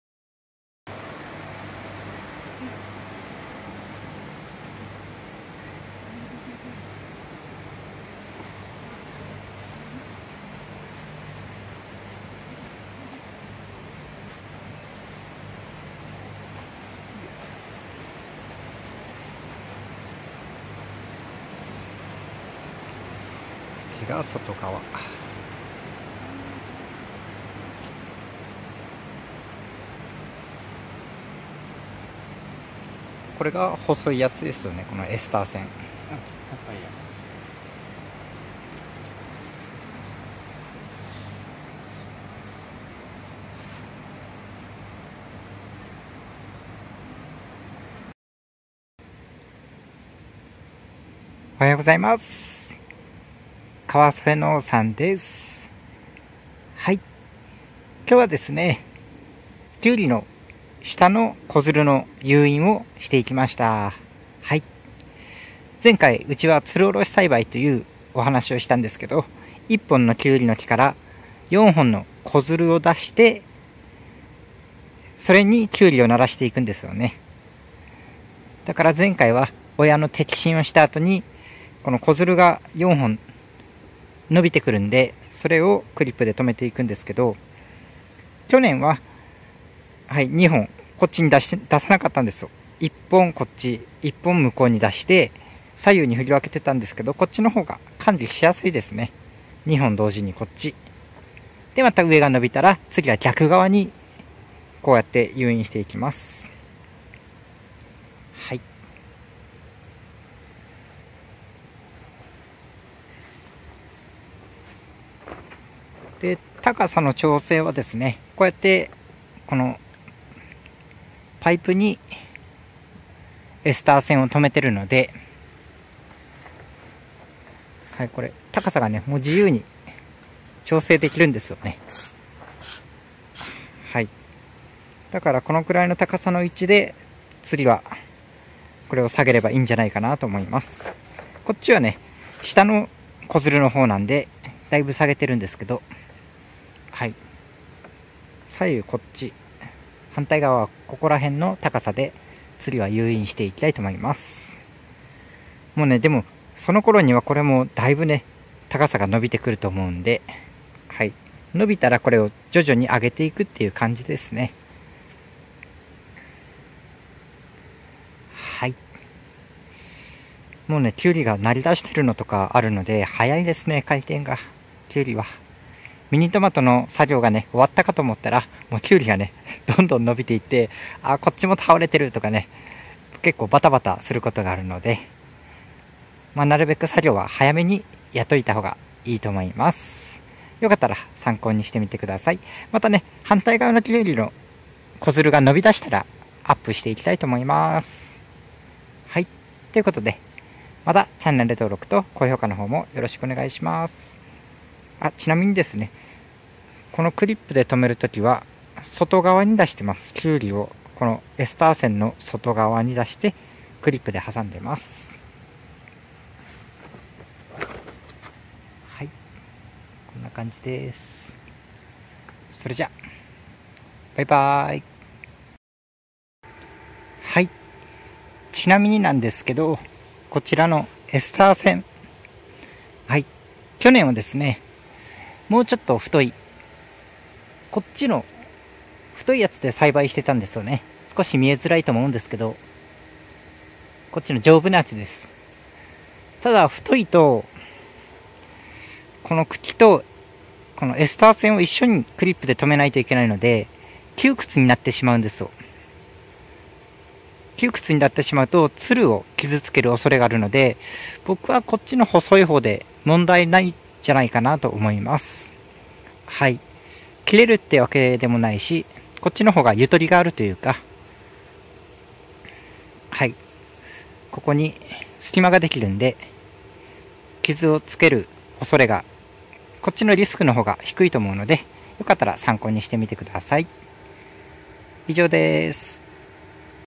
きゅうりの子づるを上手く振り分けるコツ・４本仕立てつる下ろし 2021/10/13
今回は下から伸びてきたきゅうりの子づるを上手く振り分けていきます(’-’*)♪
ただ１つ注意点があって振り分けるときに茎をねじらないように気をつけて!!